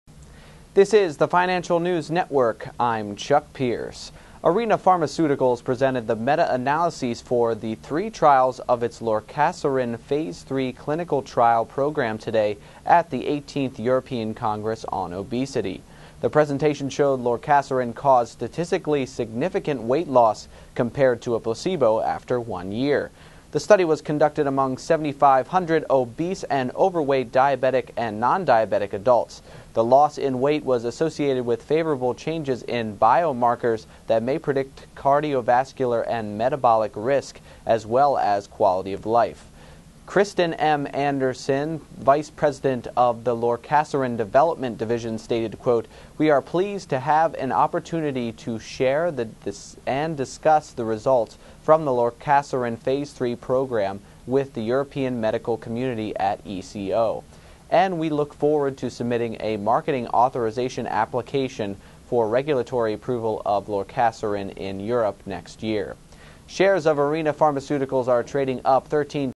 Arena Pharmaceuticals Jumps 14% On Positive Lorcaserin Phase 3 Results
Arena Pharmaceuticals, Inc. (NASDAQ:ARNA) presented the meta-analyses for the three trials of its Lorcaserin phase 3 clinical trial program today at the 18th European Congress on Obesity. The presentation showed Lorcaserin caused statistically significant weight loss compared to a placebo after one year.

The study was conducted among 7,500 obese and overweight, diabetic and non-diabetic adults. The loss in weight was associated with favorable changes in biomarkers that may predict cardiovascular and metabolic risk as well as quality of life.

Christen M. Anderson, M.D., Ph.D., Arena's Vice President, Lorcaserin Development, stated, "We are pleased to have the opportunity to share and discuss the results from the Lorcaserin Phase 3 program with the European medical community at ECO, and we look forward to submitting a Marketing Authorization Application for regulatory approval of Lorcaserin in Europe next year."

Shares of Arena Pharmaceuticals are trading up 13.7% at $1.57.